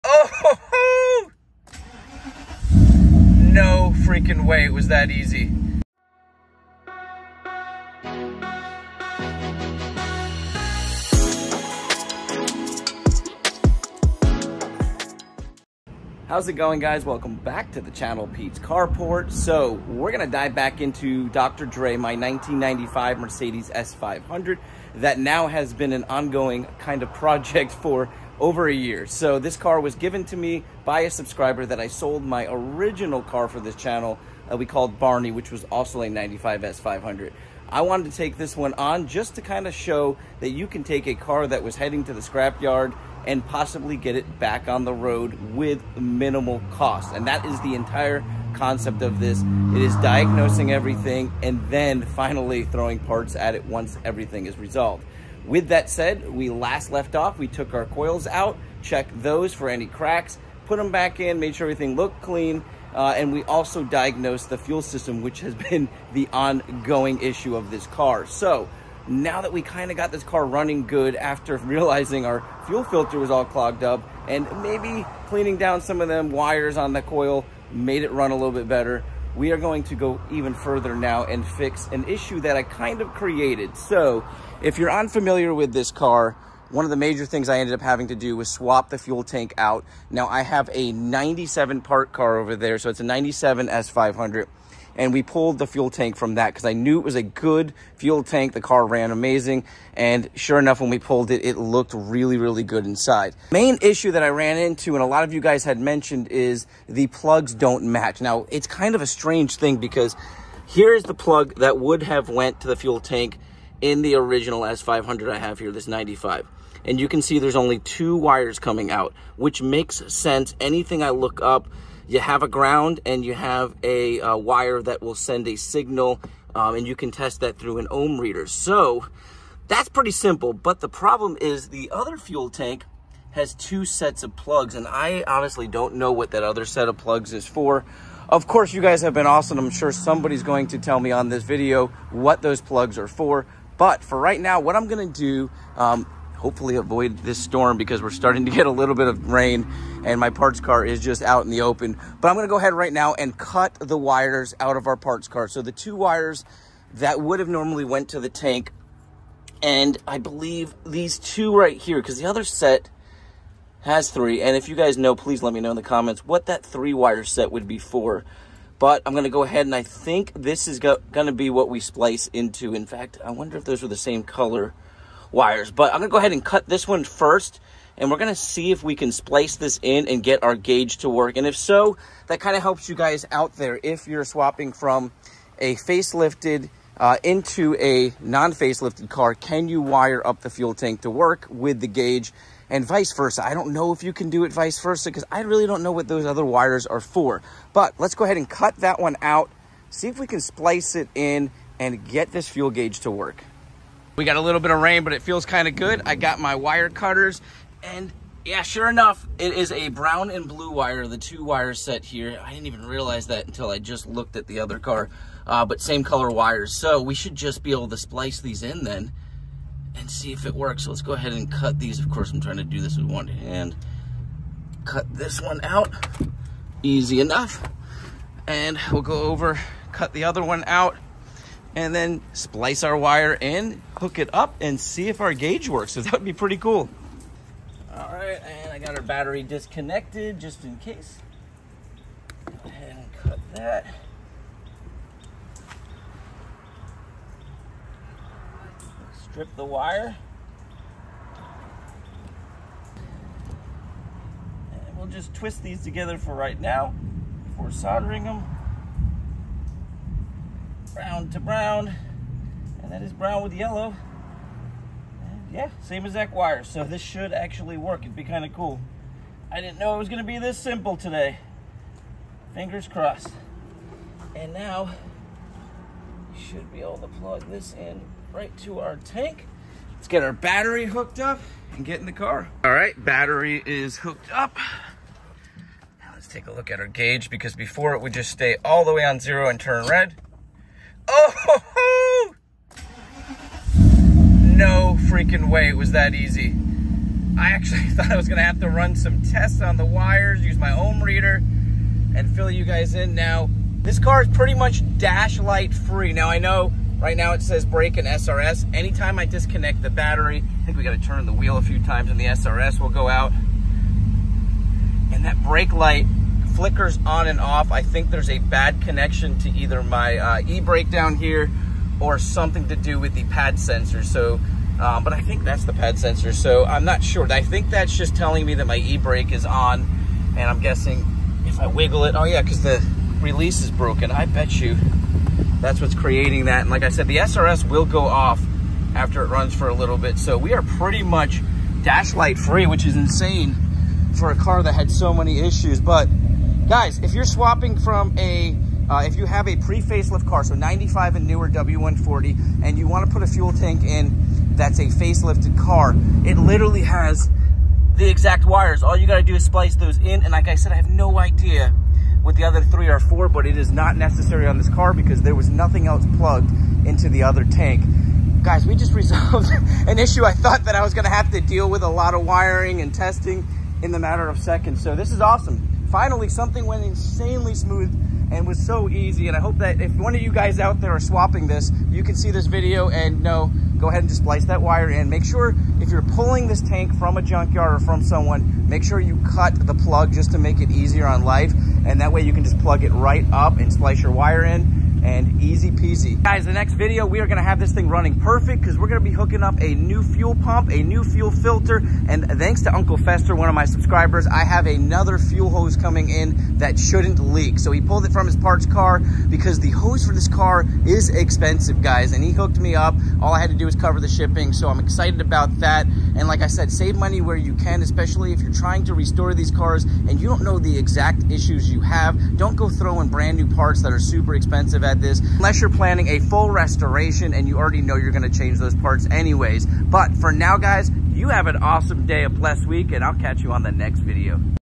Wiring fuel gauge from a facelift w140 gas tank into a pre facelift 1995 s500 project dr dr pt 8
So last year I swapped a 97 s500 gas tank from our parts car into our 1995 s500.  Issue was the wire harness from the pre facelift tank did not match up to the facelift tank.   This ended up being a very simple mod with only two wires needing to be spliced into plug.